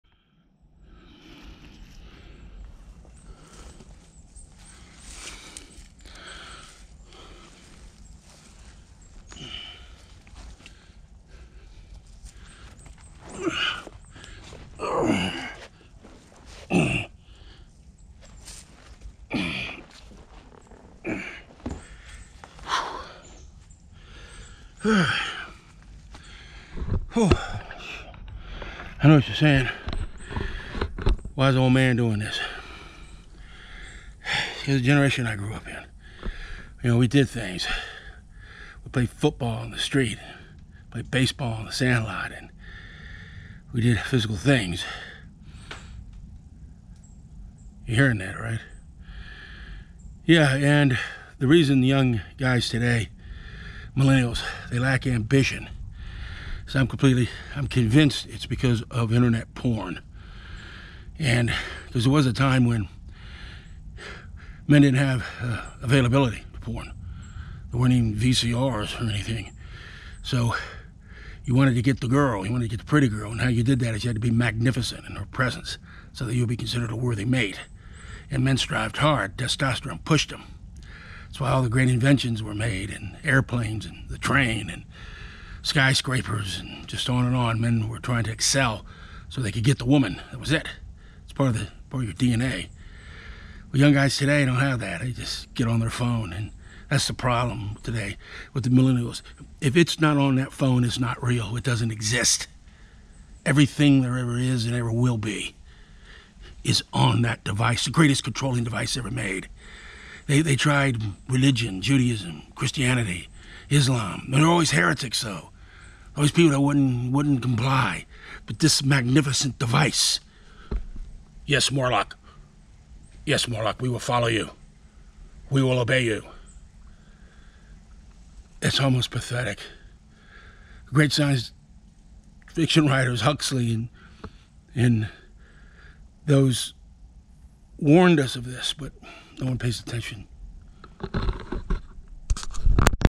Bigfoot, Internet Porn, and MILLENNIALS (4K)
Bigfoot is dominated by many of us "old guys." The reason is that Millennial men have been contaminated by pornography. Their motivation to "win the girl" has been stunted.